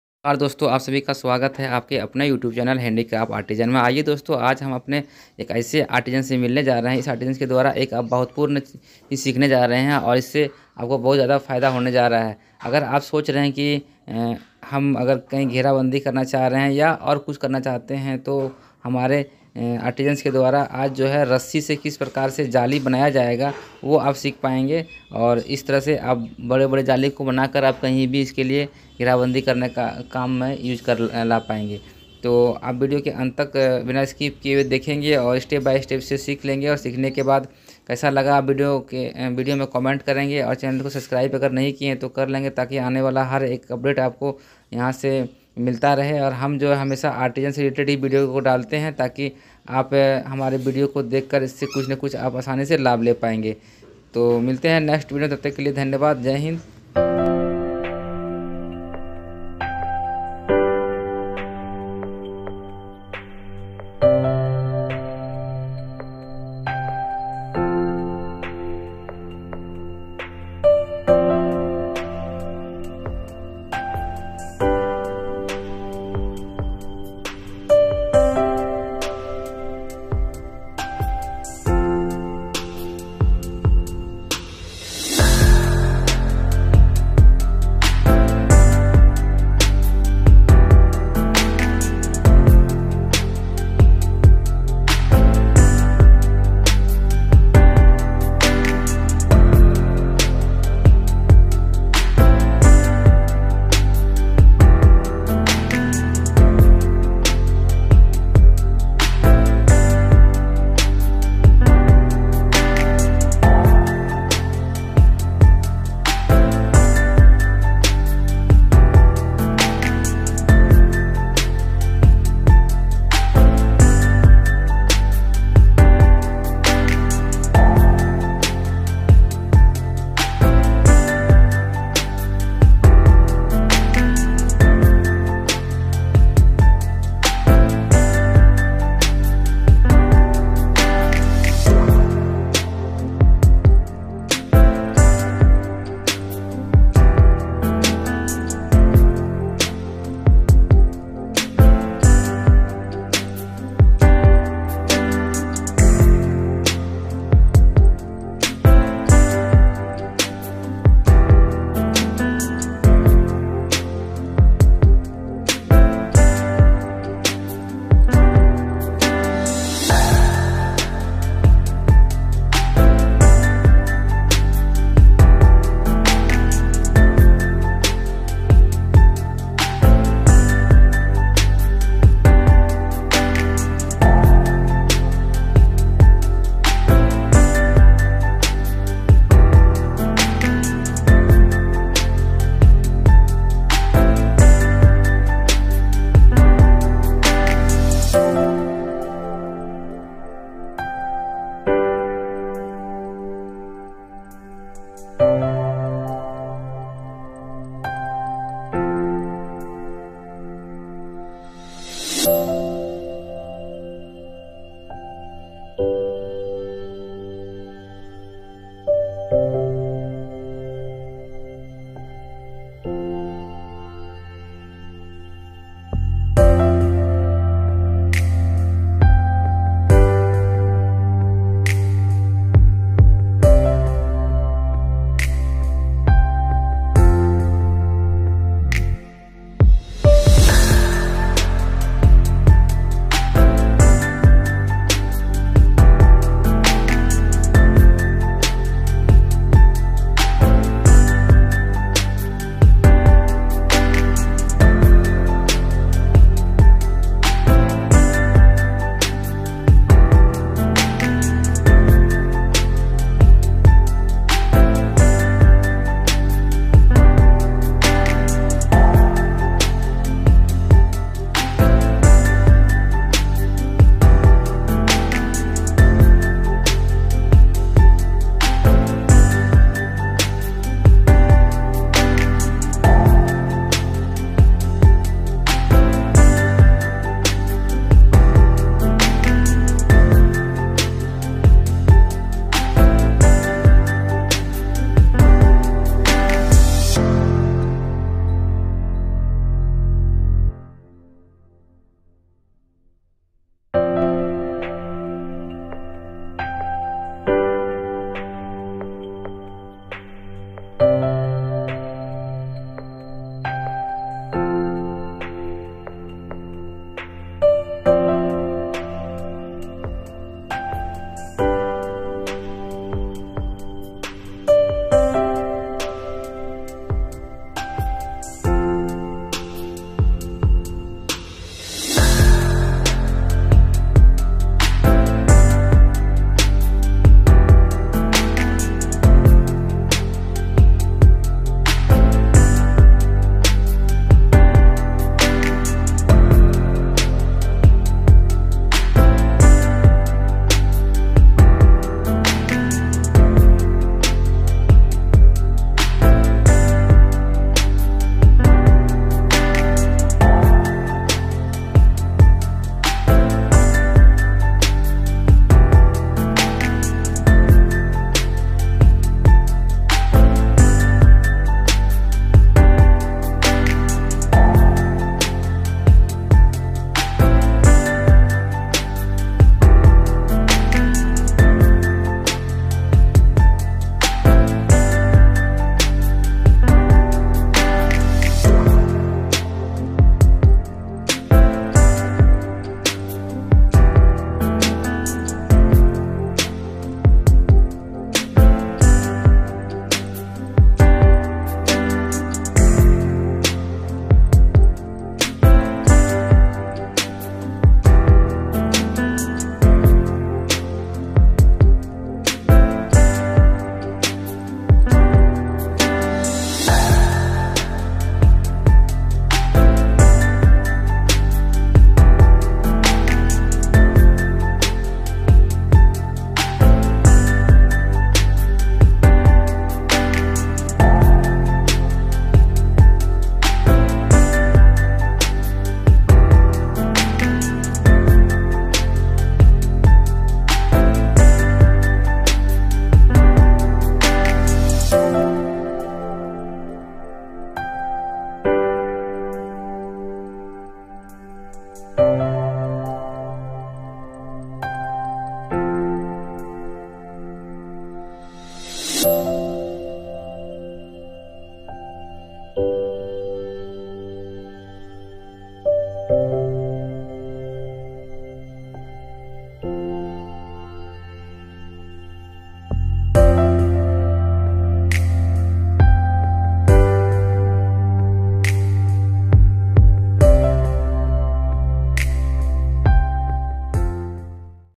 रस्सी से घेराबंदी के लिए जाली बनाने सीखें
आइए रस्सी से हम लोग बनाने सीखते हैं जाली
घेराबंदी करने के लिए जाली बनाना सीखे
घेराबंदी करने के लिए रस्सी से जाली बनाना सीखें
एक बार इस वीडियो को देख लें आप हमेशा के लिए सीख जाएंगे रस्सी से जाली बनाना
बस एक बार आप को ध्यान देना है और जिंदगी भर याद रखना है इस प्रकार से जाली बनाया जाता है